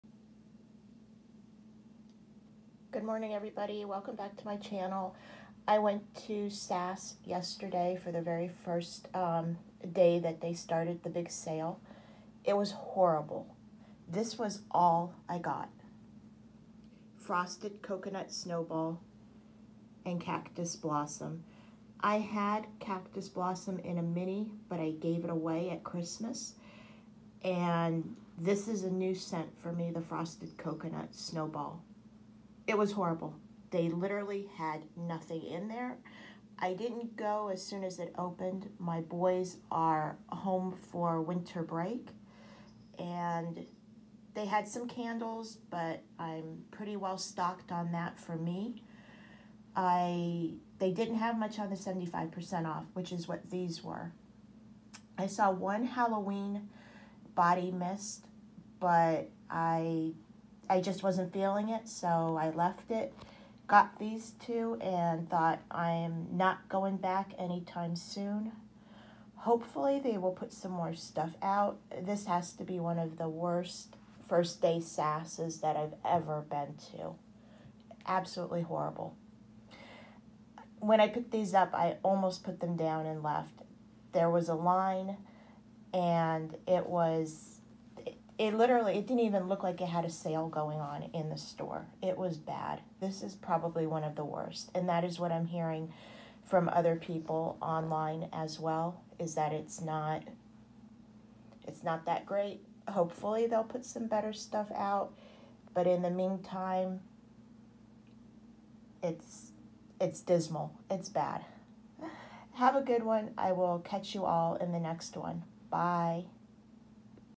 First Day of SAS winter 2023!!!
It was horrible!!!  I couldn’t go when they first opened due to my boy’s being home for Christmas break.  I will go back but not soon!